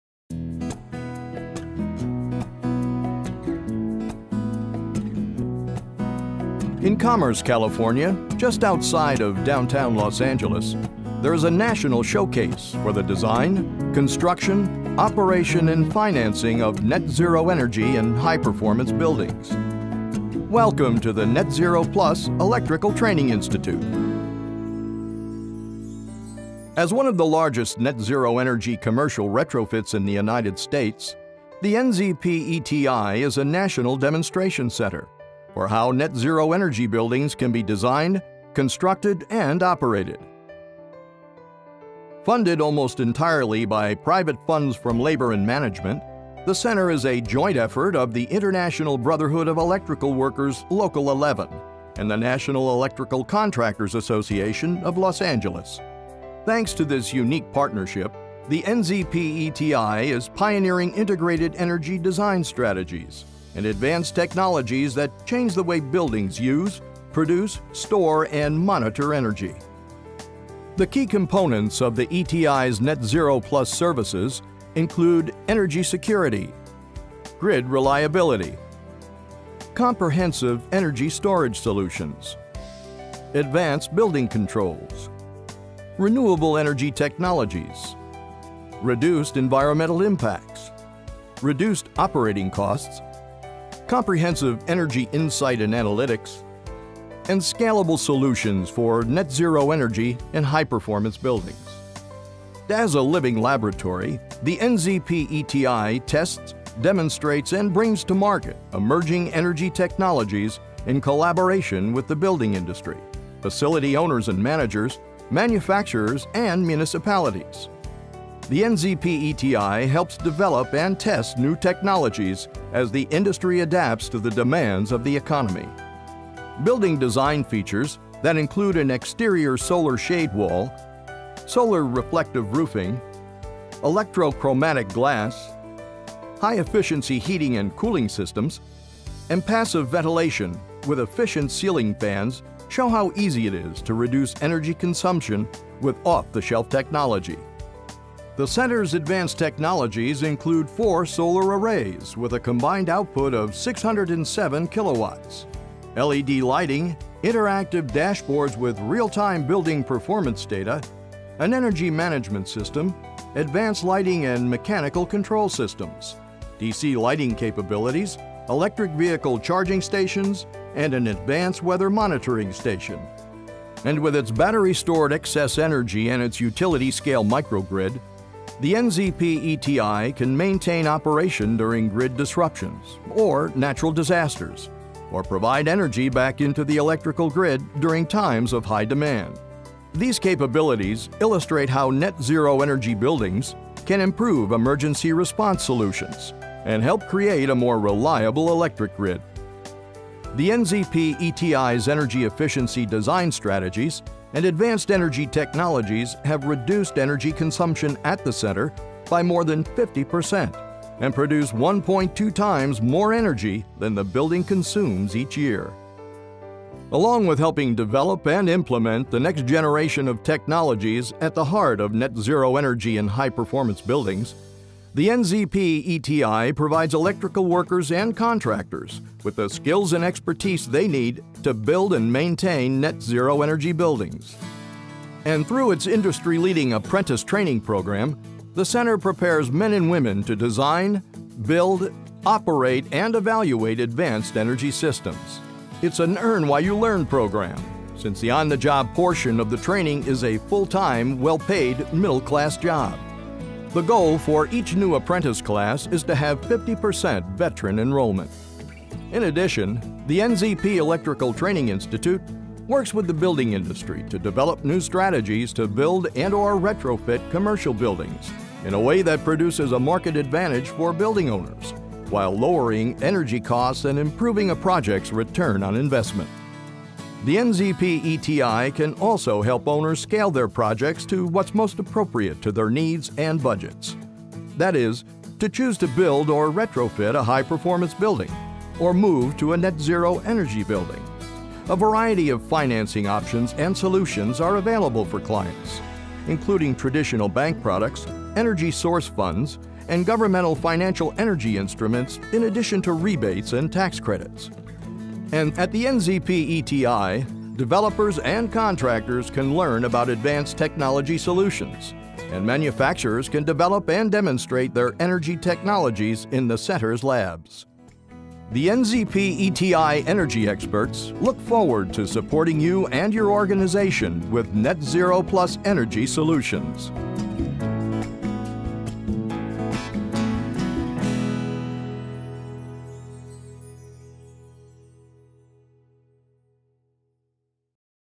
ETI NetZero Plus Introduction Video
Recognized as the premier electrical training school in the nation, the Net Zero Plus Electrical Training Institute in Los Angeles is a partnership of the International Brotherhood of Electrical Workers Local 11 and the Los Angeles Chapter of the National Electrical Contractors Association. Each year, we train more than 2,000 electrical apprentices at our state-of-the-art electrical training center to be prepared for challenging and rewarding careers in the electrical industry.

The Net Zero Plus Electrical Training Institute is the largest net zero energy, commercial retrofit in the U.S. It is a national demonstration center for how net zero energy buildings should be designed, constructed, and operated. The NZP ETI has been designed as a living laboratory to test, demonstrate, train, and bring-to-market emerging energy technologies. Through the living laboratory, we work closely with the building industry, facility owners and managers, manufacturers, and municipalities. The NZP ETI continually evolves with new technologies as the industry continues to grow under the new energy economy.